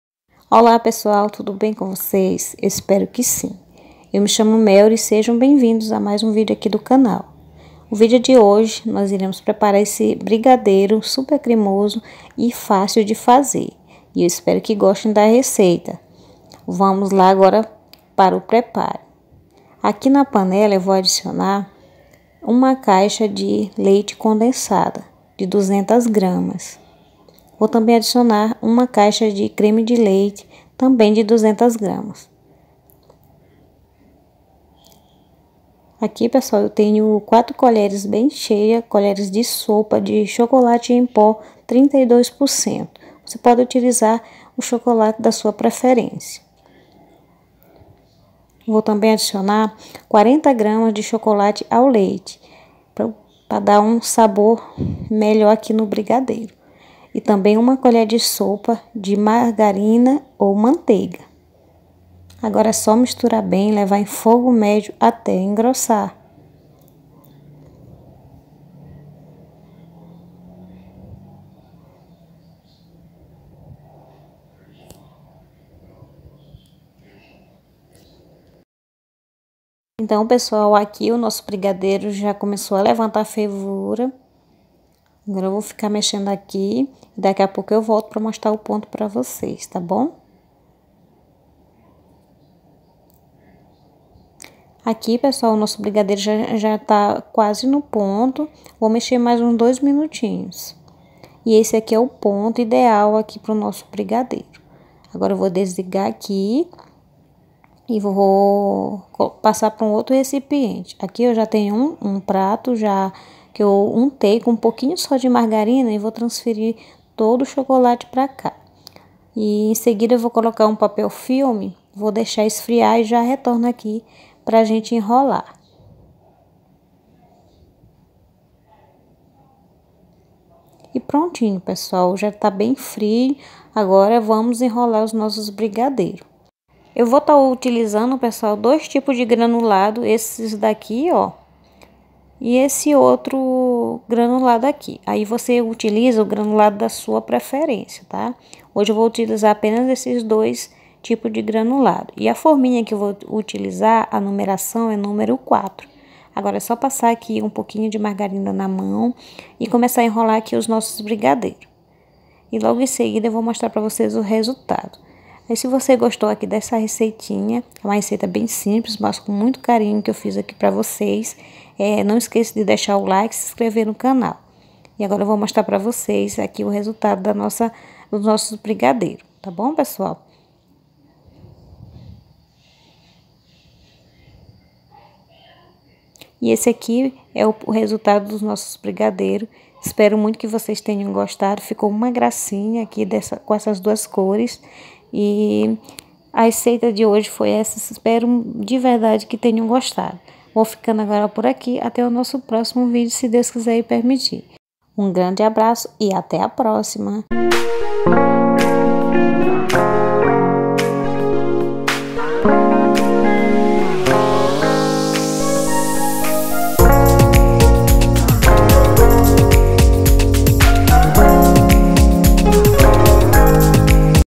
⭐BRIGADEIRO DE CHOCOLATE//SIMPLES E FÁCIL
Oi gente!
Quem aí gosta de brigadeiro?,impossível não gostar né, por isso trago essa receita simples e fácil de fazer para compartilhar com vocês, espero que gostem!
👉📝Anota aí os ingredientes 👇
1 caixa de creme de leite de 200g
1 caixa de leite condensado de 200g
1 colher de sopa de margarina //Manteiga
40g de chocolate ao leite (opcional )
4 colheres de sopa de chocolate em pó 32%
👉forminha número 04